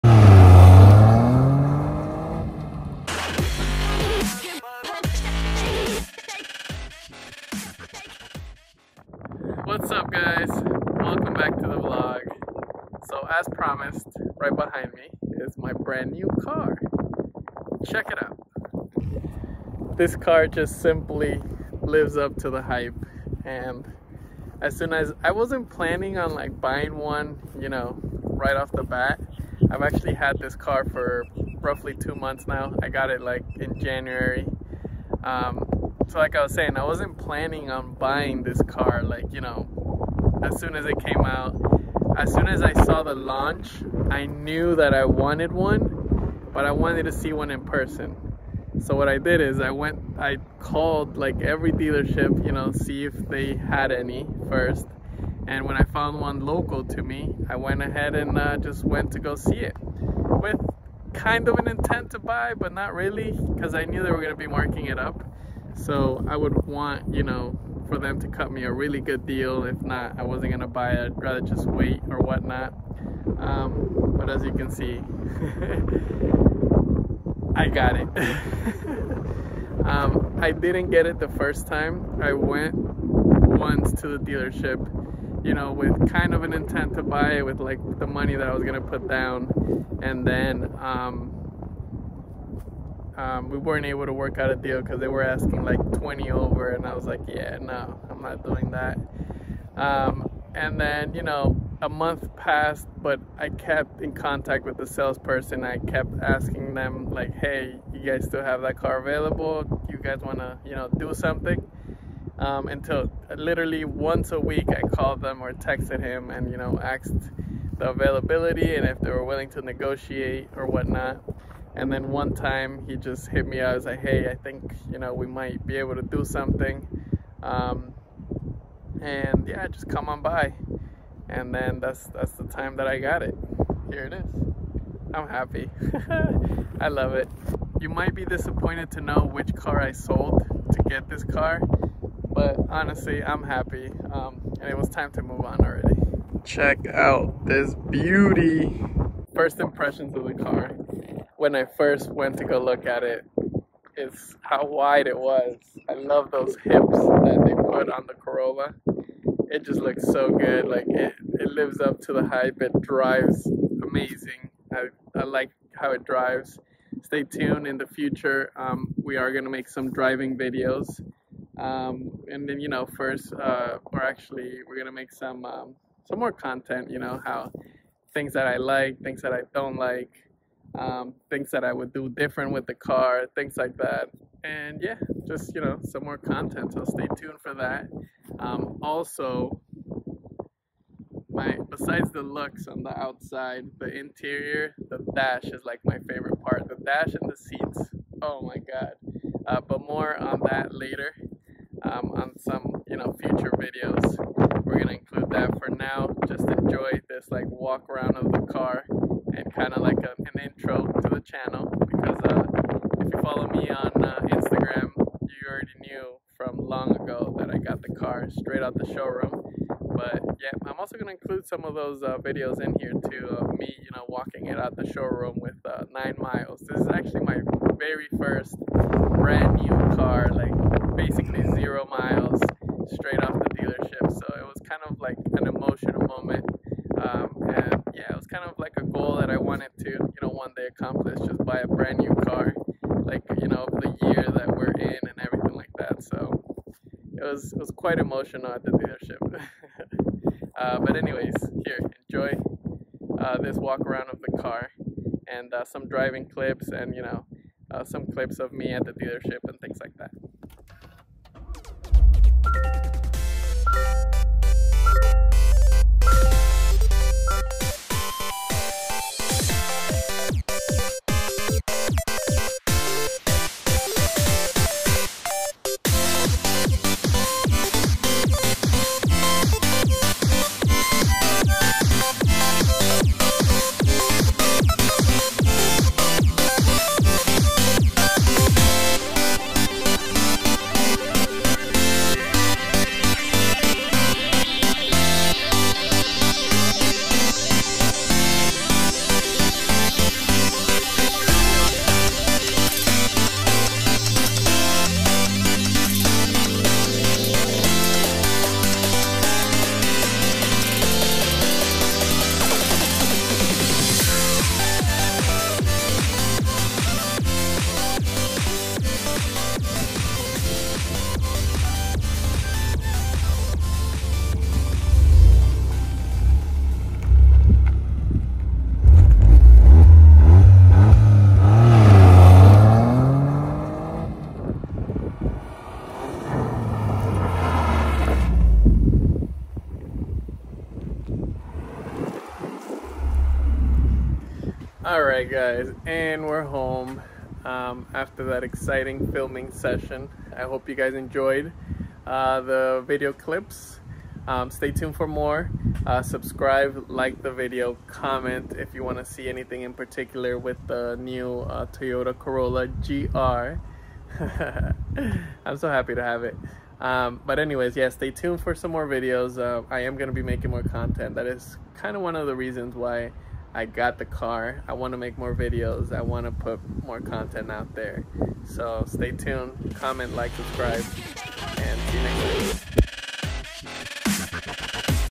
I Bought a 2023 Toyota Corolla GR!
In todays Vlog, we take a look at my new car! Ive had this car for two months now but i have not had time make a video about it. I hope you enjoy, more content to come!